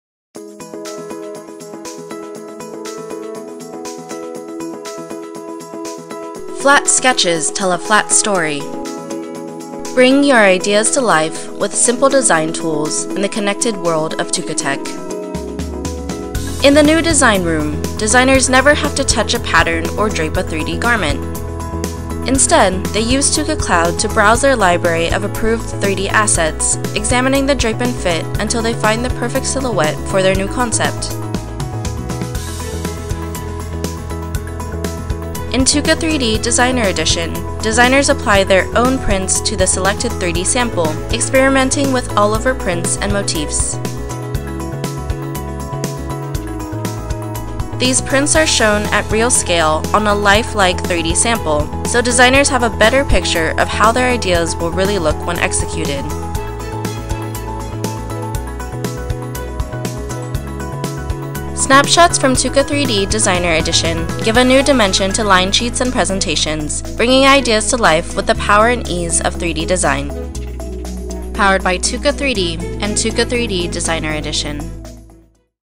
"Flat Sketches Tell a Flat Story" - The New Linesheet | Demonstration | TUKA3D Designer Edition
Flat sketches tell a flat story. Bring your ideas to life with our 3D visualizer's simple design tools in the connected world of Tukatech.  No pattern-making or draping required!